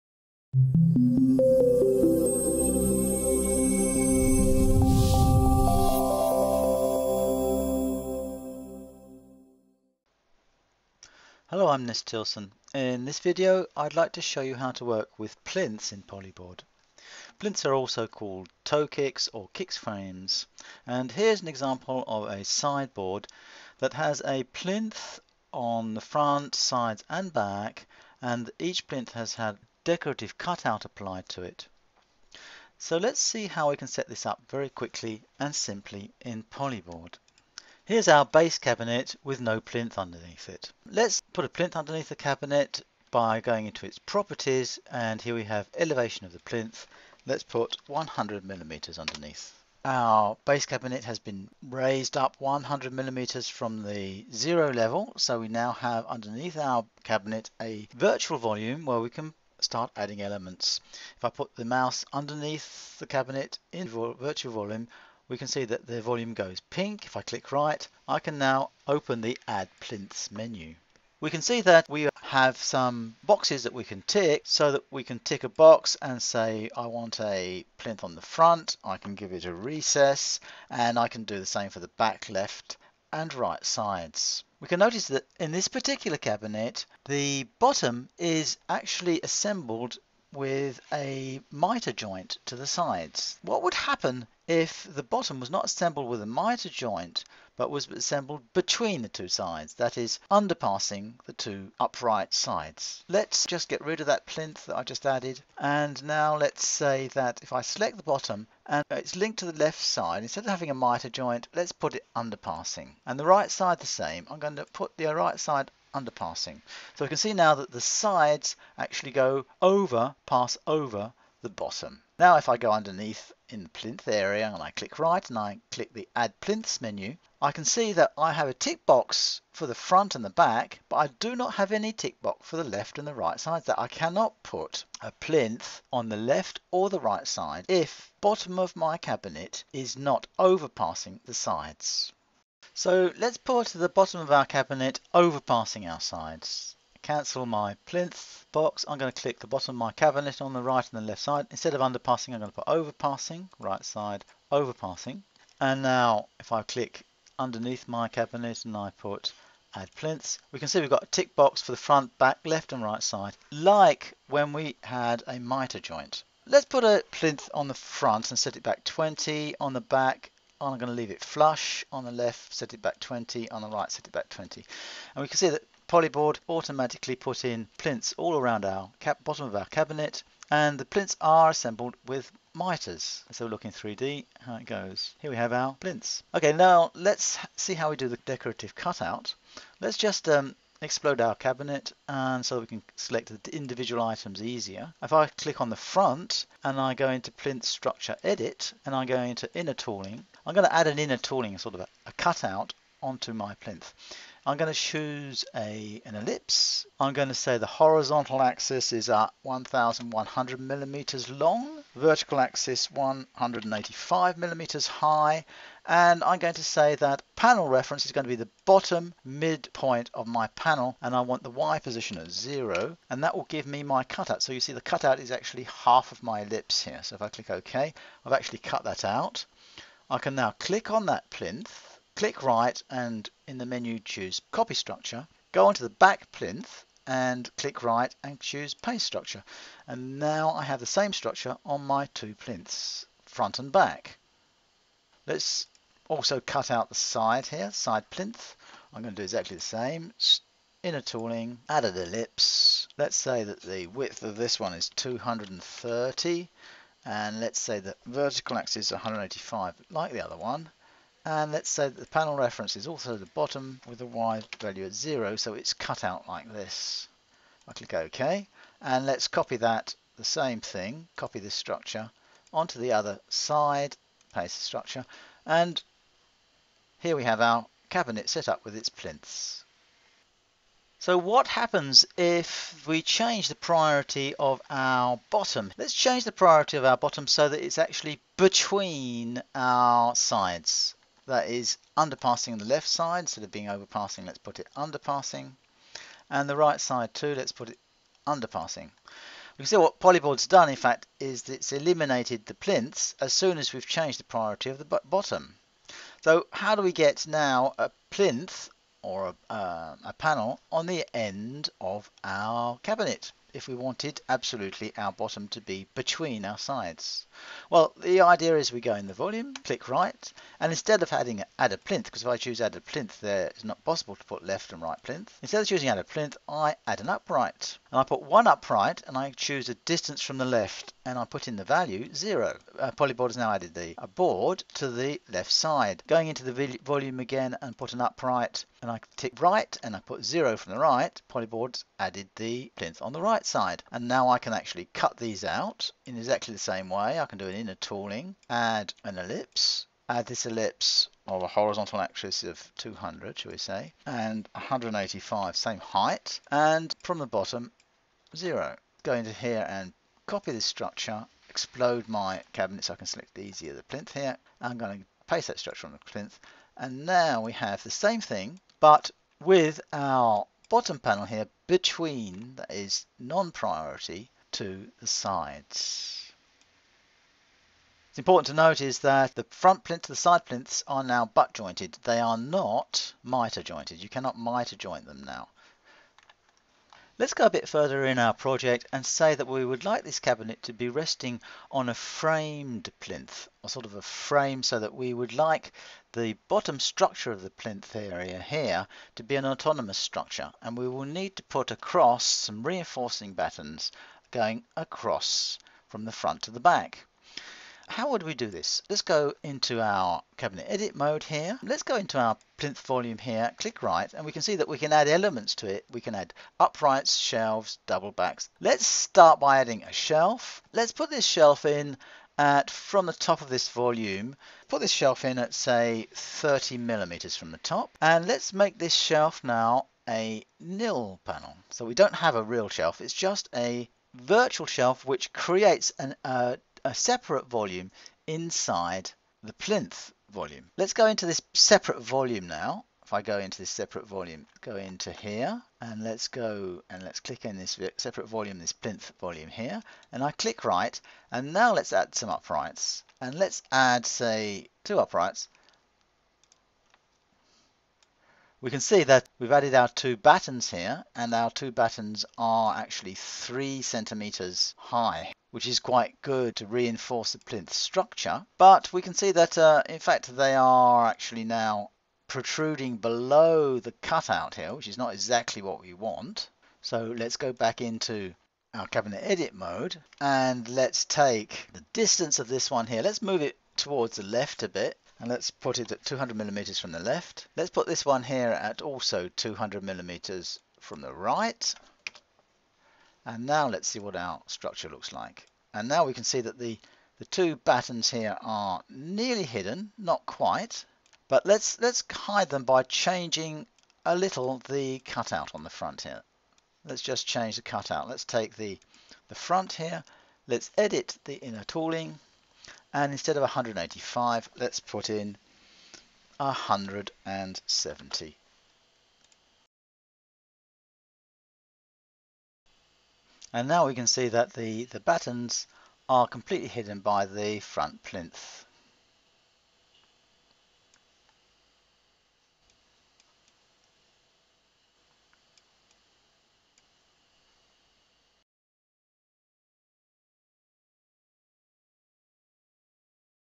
How to Work With Plinths in Polyboard
This video explains how to design furniture in Polyboard that uses plinths, toe kicks, and kick frames.